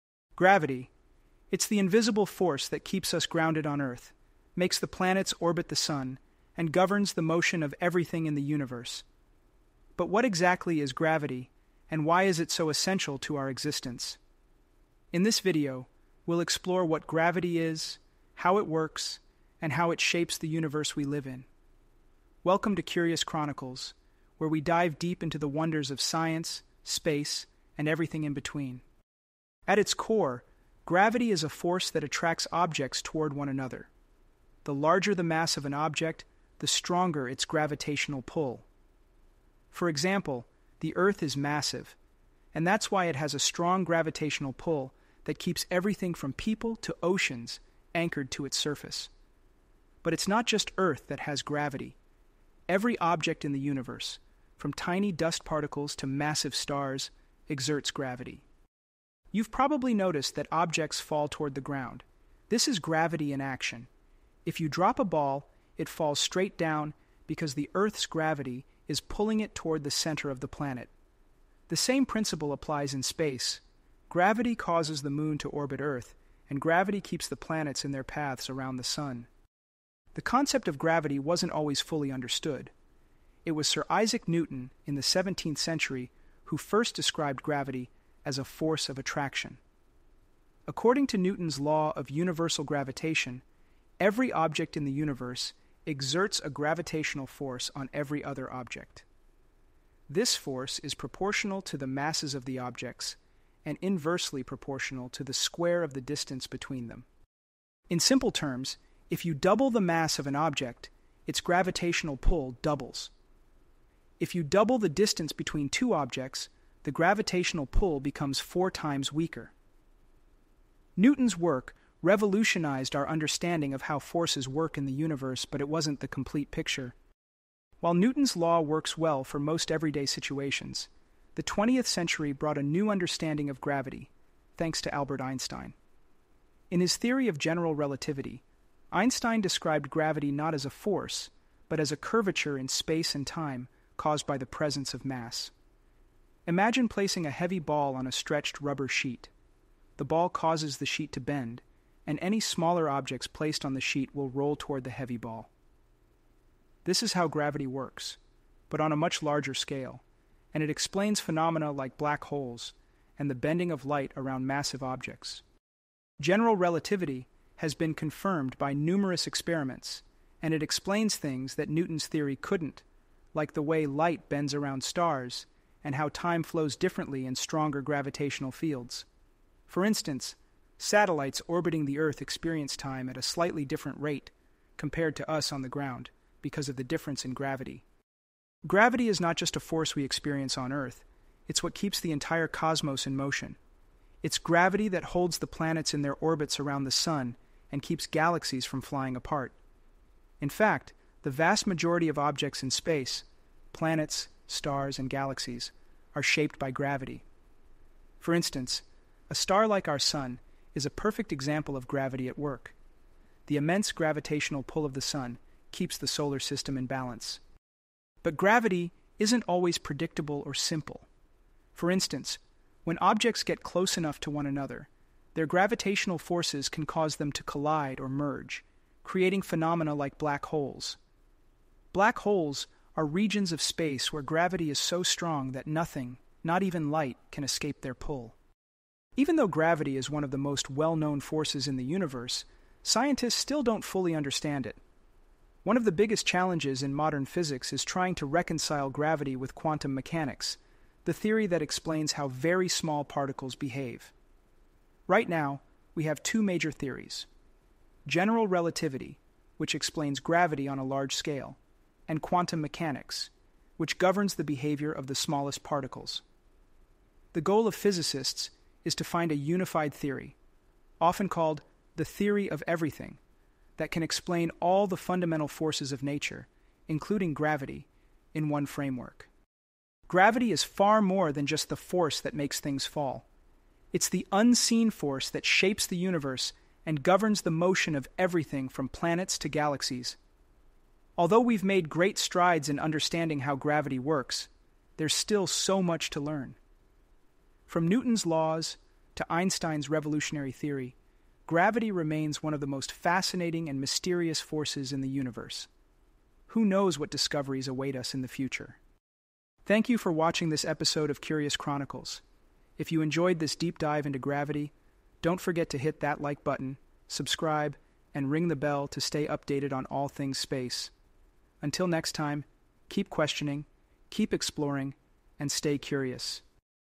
What is Gravity? Unraveling the Mystery of the Force that Shapes the Universe
In this video, we explore one of the most fundamental forces in nature: gravity. From the fall of an apple to the motion of galaxies, gravity shapes our universe in ways we can’t always see. Join us as we dive into the history of gravity, from Newton’s laws to Einstein’s theory of general relativity, and learn how this invisible force affects everything from the planets to the very fabric of space-time itself.

Stay curious and keep exploring the wonders of space with Curious Chronicles.